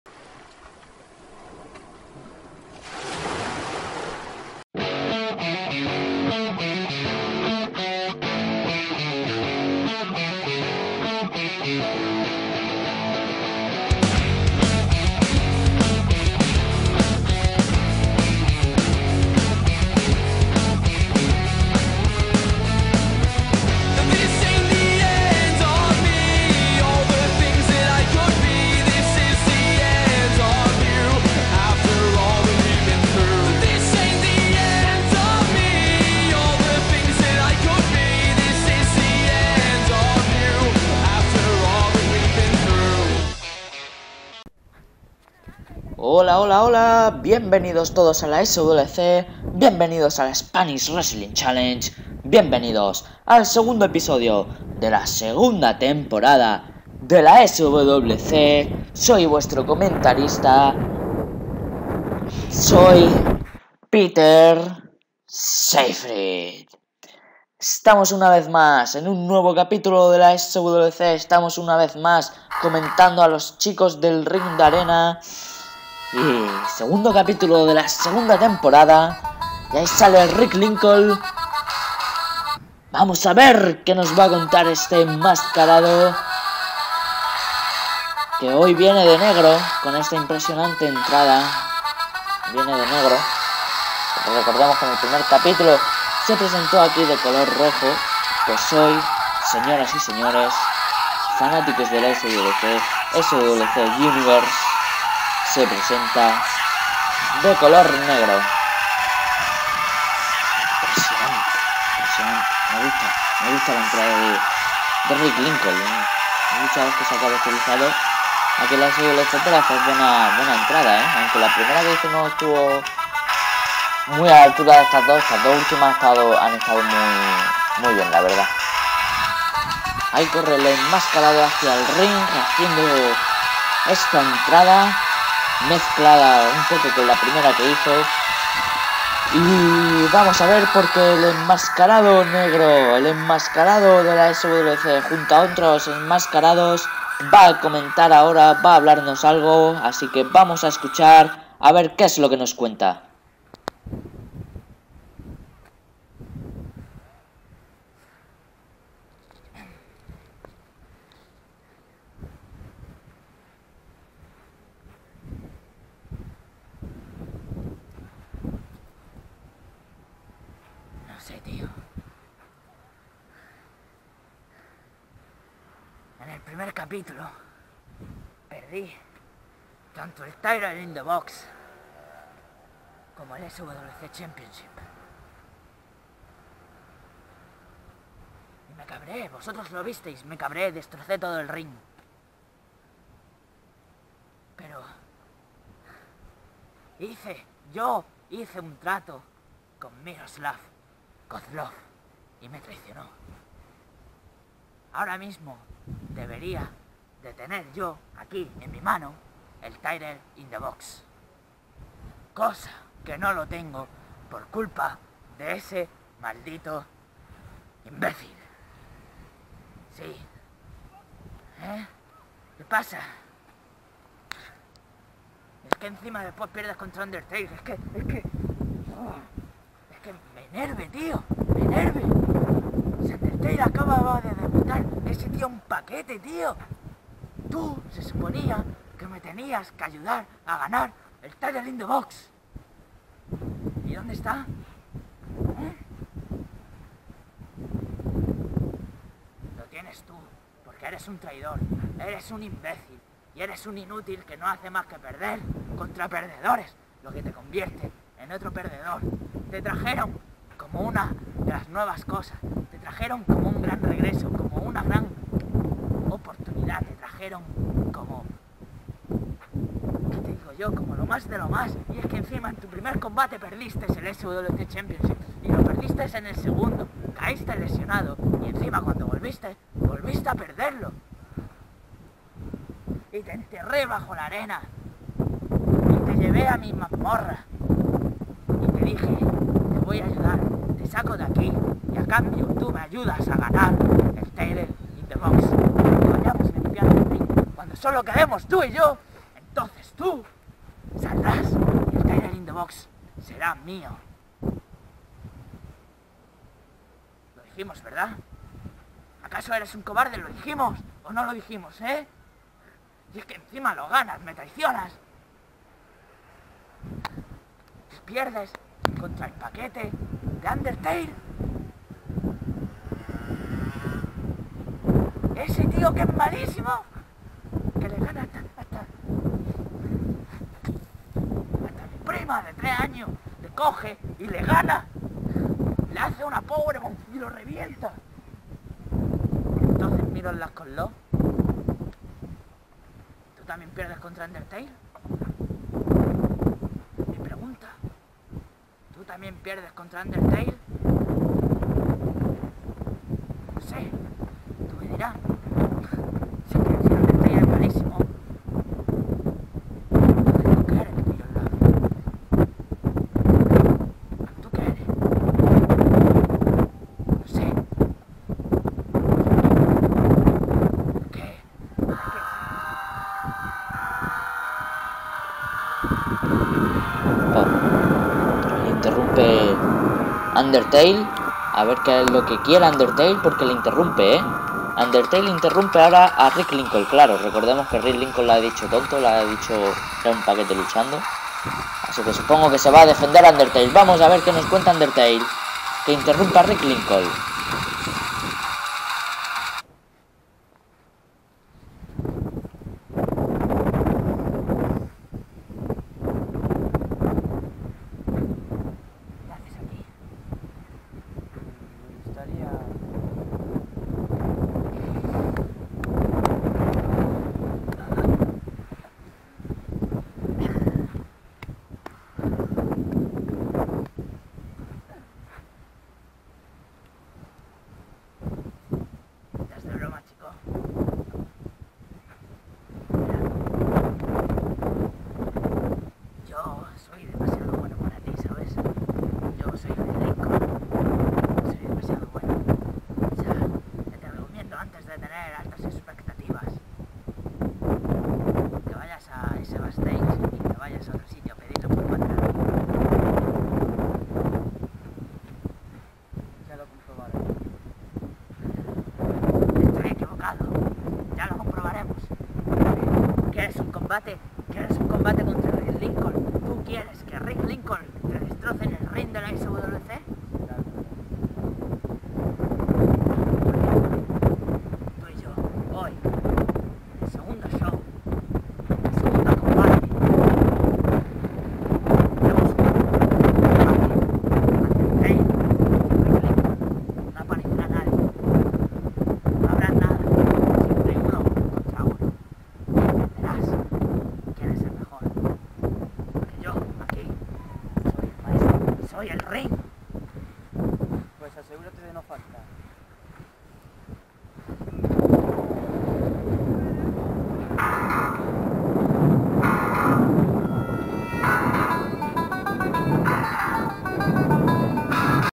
SWC 2x02 | Rick Lincoln al habla y Undertale interrumpe y le reta a un combate | Backyard Wrestling
Regresa la SWC con este segundo capítulo de la temporada tras el primer capítulo emocionante. En esta primera parte, Rick Lincoln sale a hablar sobre todo lo ocurrido y perdido en el primer episodio de la temporada como el Title in the Box o el SWC Championship, y, además, Undertale le interrumpe en la conversación para, simplemente, retarle a un combate por algún comentario... disfruten.